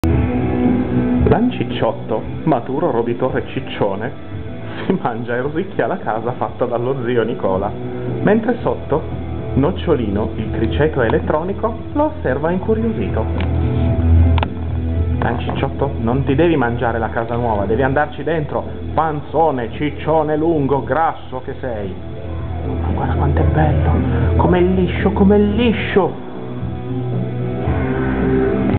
Lancicciotto e Nocciolino
Lancicciotto, maturo roditore, osserva la nuova casa in compagnia di Nocciolino, il criceto elettronico